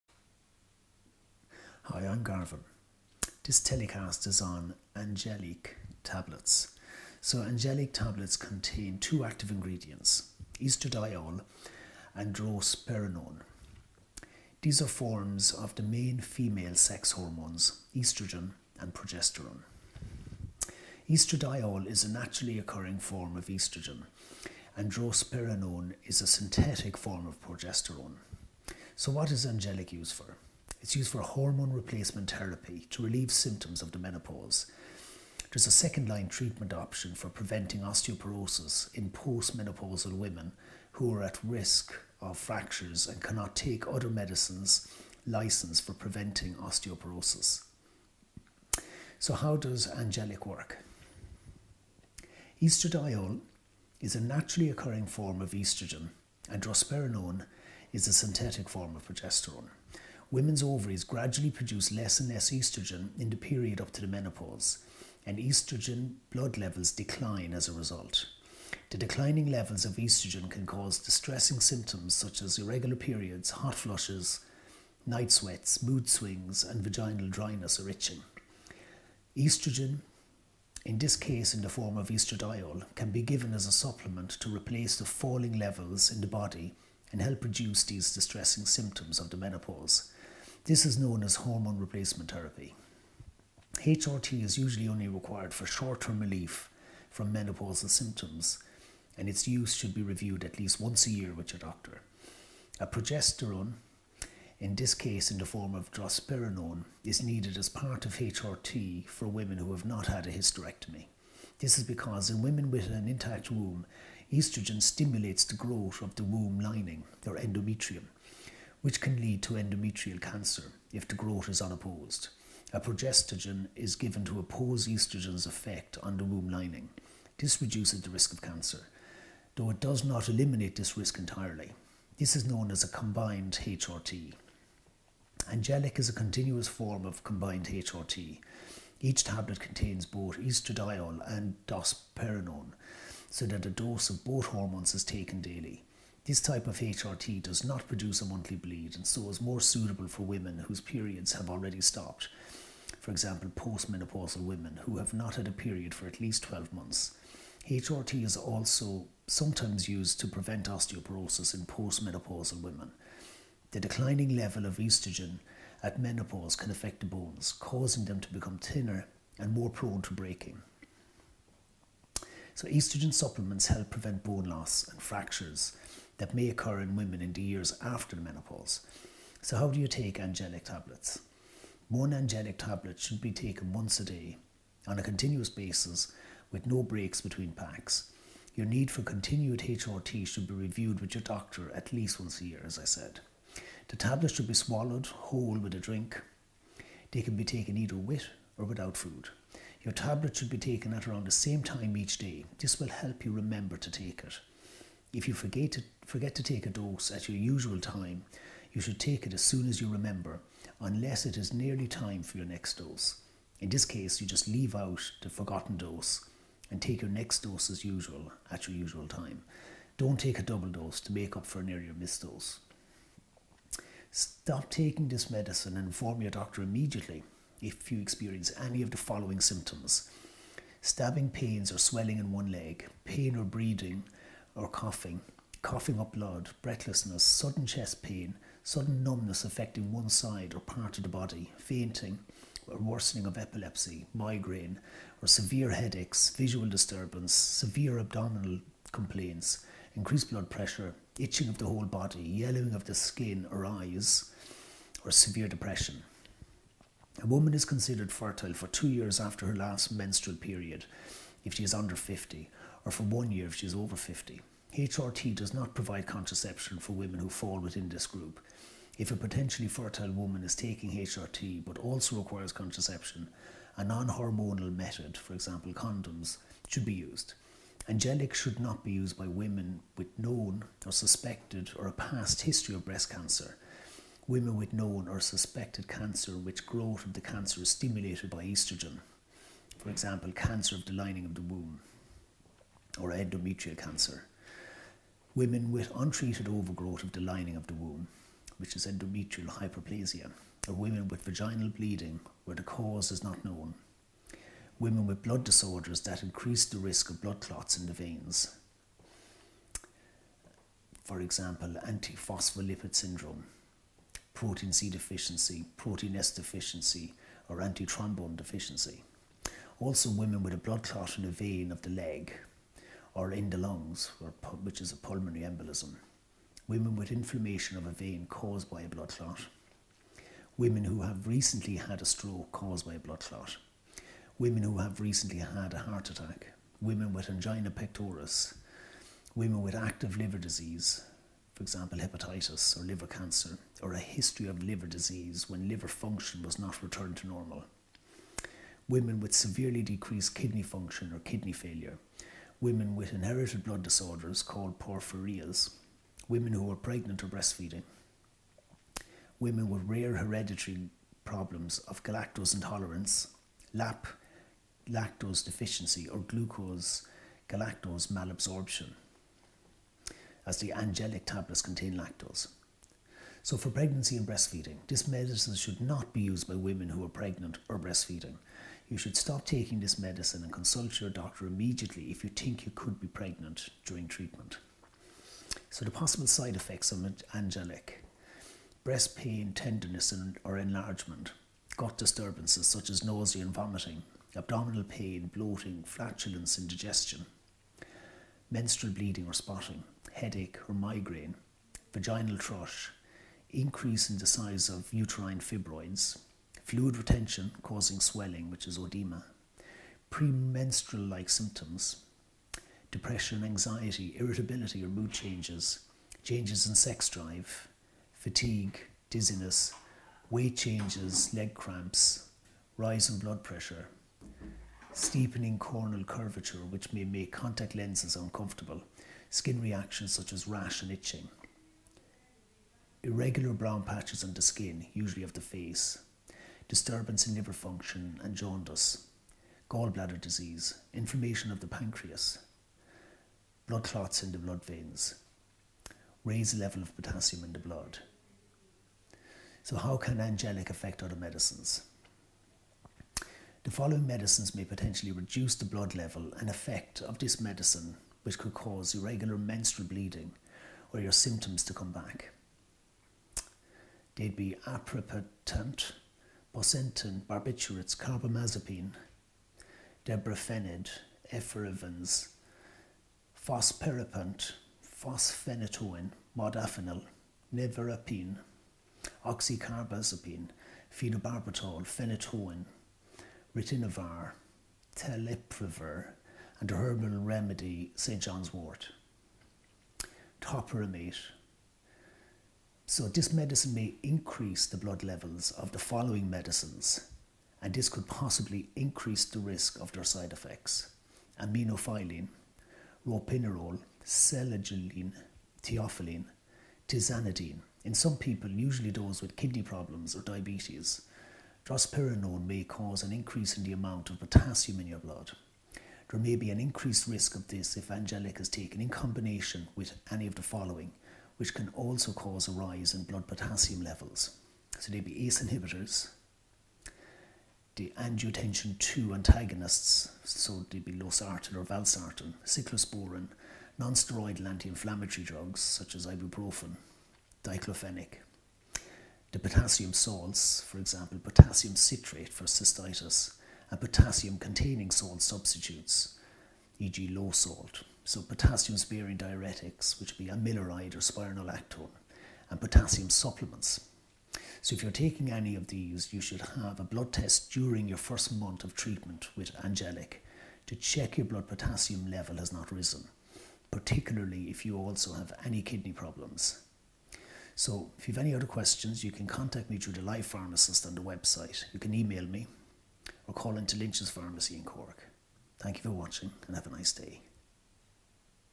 Angeliq tablets
Angeliq tablets contain two active ingredients, estradiol and drospirenone. These are forms of the main female sex hormones, oestrogen and progesterone. Estradiol is a naturally occuring form of oestrogen and drospirenone is a synthetic form of progesterone.

You can also visit Lynch's Pharmacy in Broadale, Maryborough Hill, Cork.